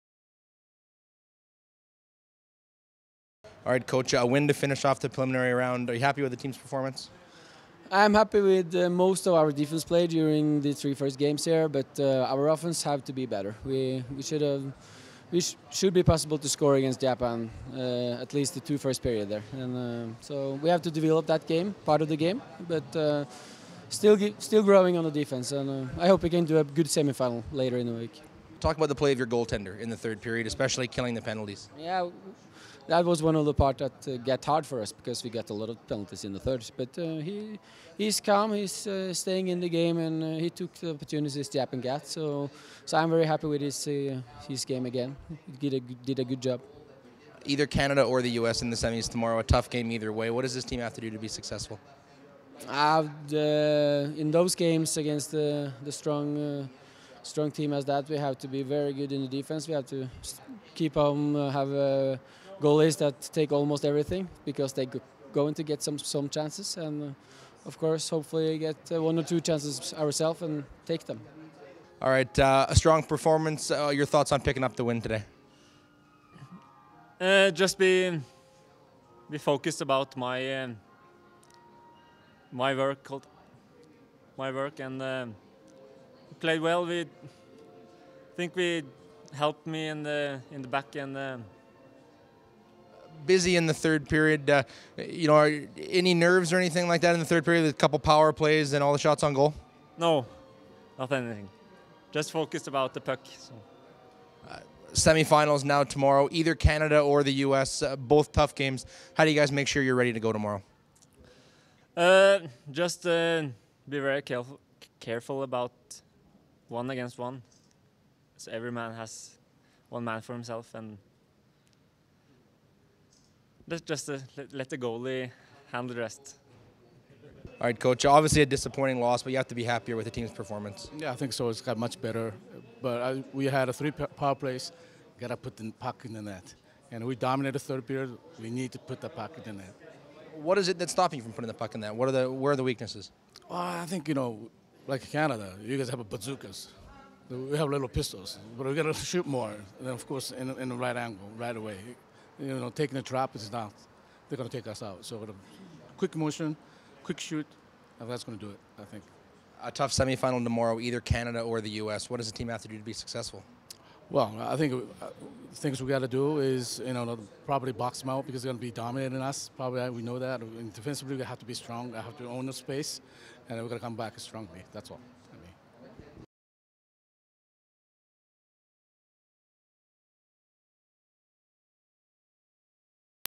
WSHC Post-Game: NOR 1, JPN 0 (SO) (Dec. 5)
Reaction from Norway's 1-0 preliminary round shootout win over Japan at the 2012 World Sledge Hockey Challenge on Dec. 5. Audun Bakke and Morten Værnes scored goals in the shootout as the Norwegians claimed third in the preliminary round with a win over Japan in a game dominated by goaltenders Kjell Christian Hamar and Mitsuru Nagase.

for more information on your favourite teams and events.

Consultez le pour plus de renseignements sur vos équipes et événements favoris.